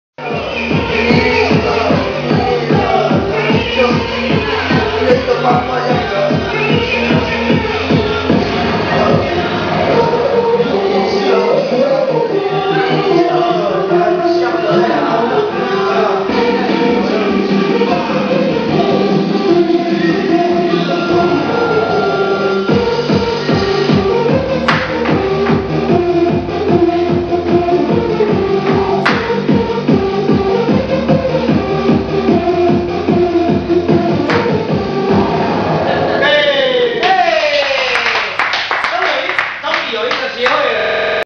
8-15恆春金牌ktv 俊廷教練-三太子舞蹈教學.wmv
俊廷教練教導tracy跳三太子.